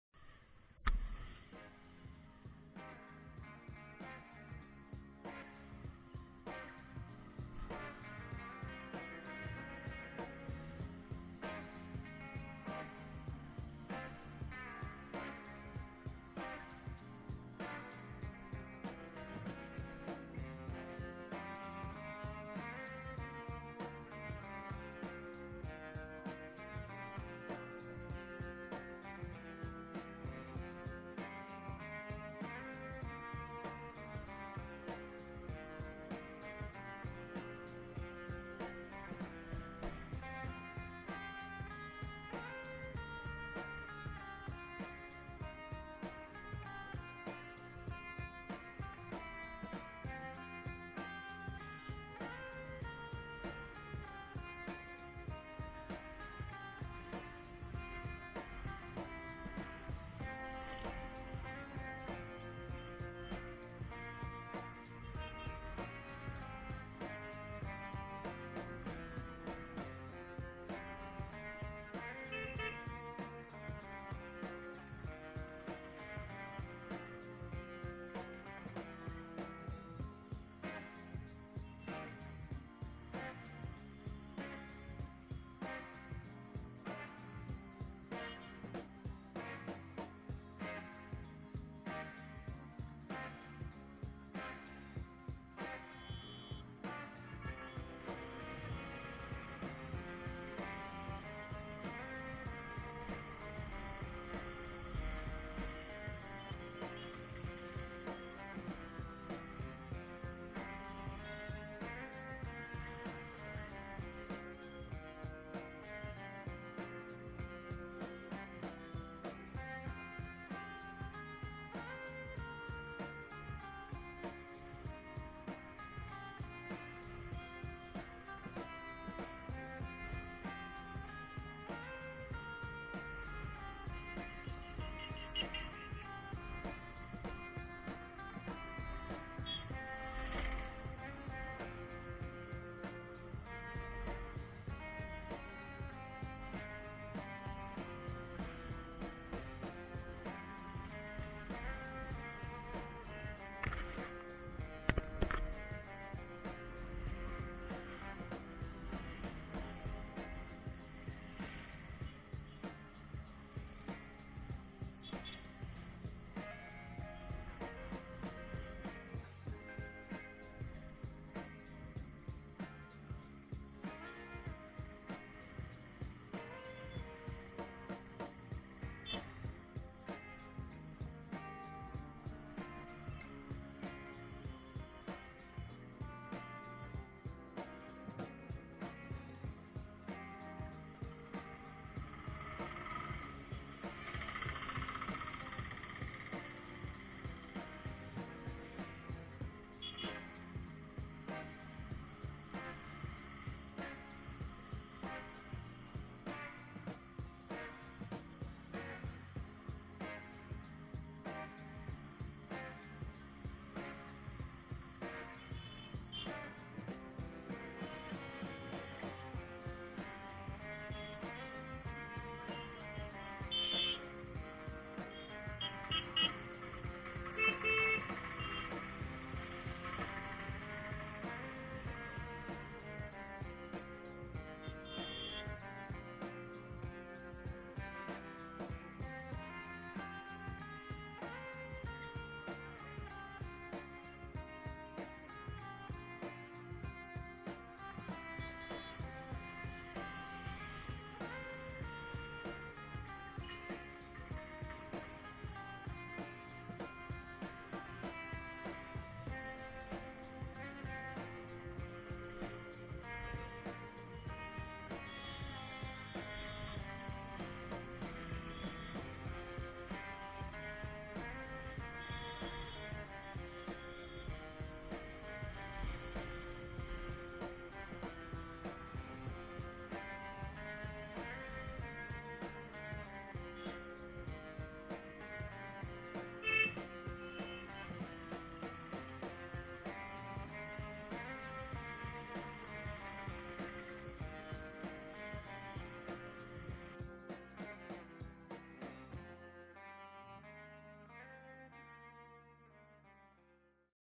Jorhat City (Thana Road)
A short ride from Vidyut Bhavan, Jorhat to Nirmal Chariali Via Thana Road, Jorhat.